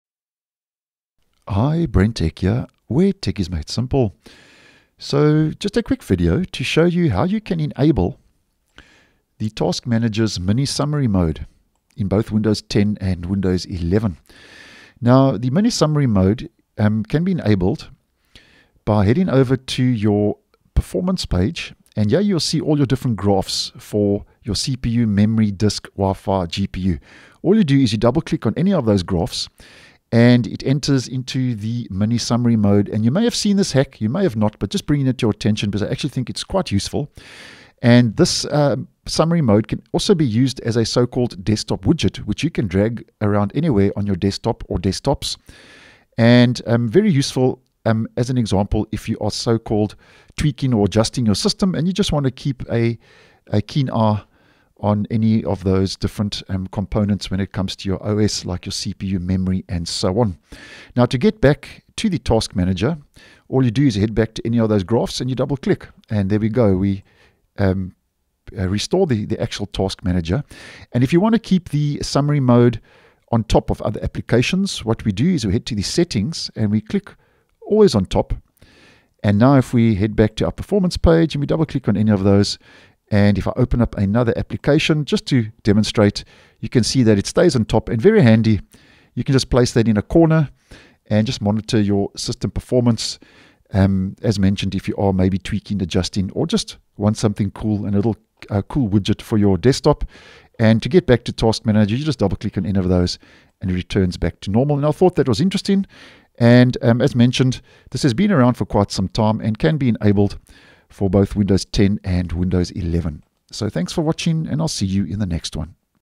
Enable Task Manager's hidden "Widget" for your Desktop
If you double click the graphs on the left side of Task Manager's performance page, it'll go into a mini summary mode.